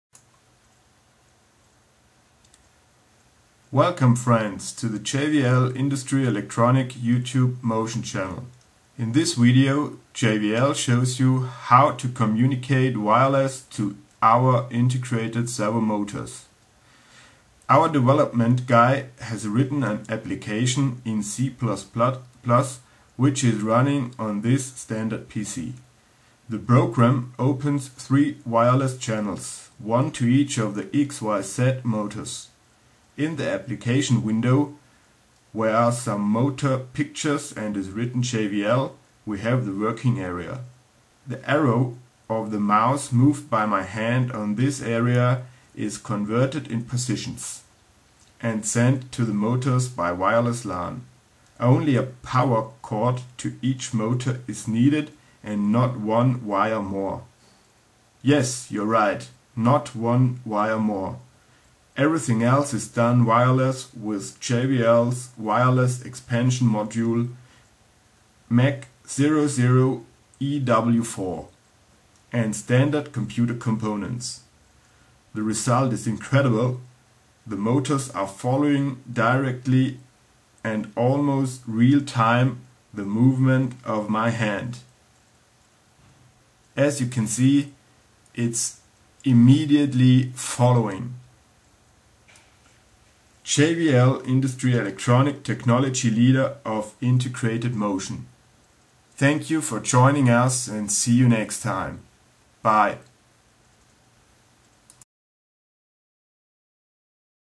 JVL integrated servo motors controlled wireless
In this video you see an X-Y-Z application with JVL integrated servo motors controlled wireless. The hand moves the arrow of the mouse on a defined area of the monitor and the motors controlled wireless are following this movement.